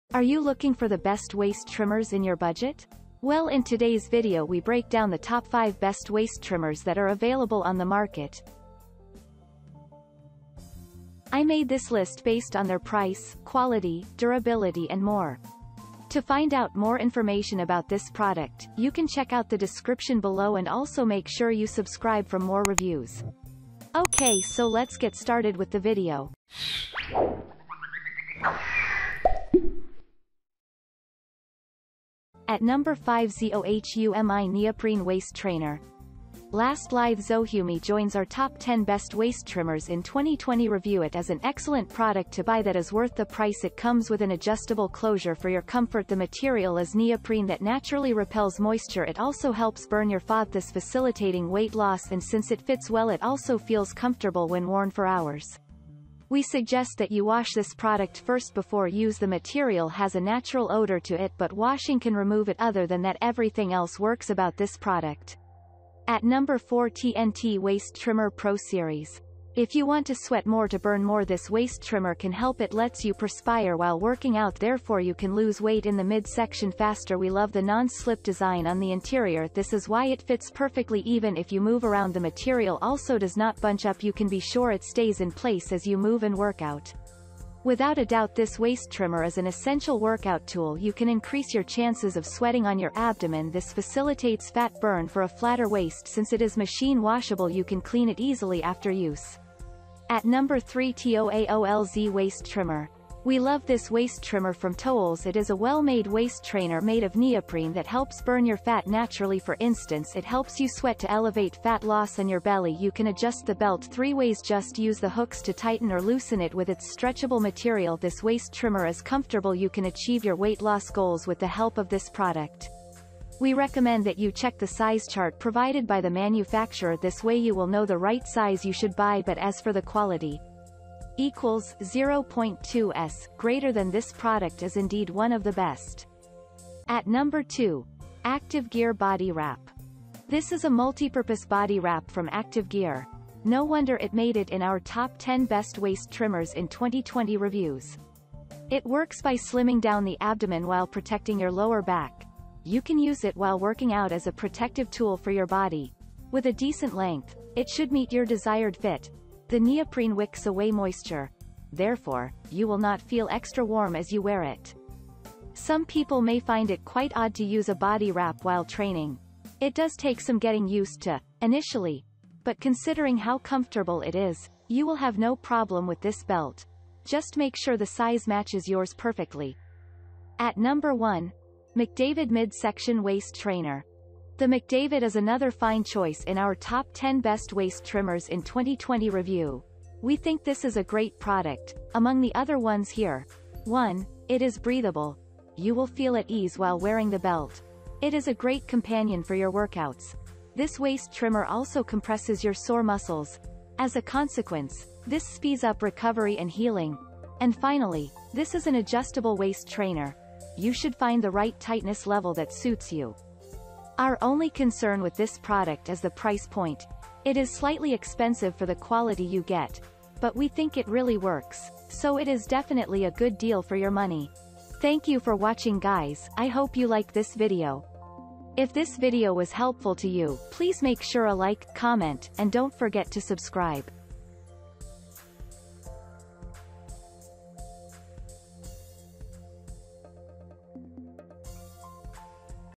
Top 5 Best Waist Trimmers in 2021 Review
☛ All The Links to Waist Trimmers Listed in this Video:-

▶️ 5. ZOHUMI Neoprene Waist Trainer.

▶️ 4. TNT Waist Trimmer Pro Series.

▶️ 3. TOAOLZ Waist Trimmer.

▶️ 2. ActiveGear Body Wrap.

▶️ 1. Mcdavid Mid-Section Waist Trainer.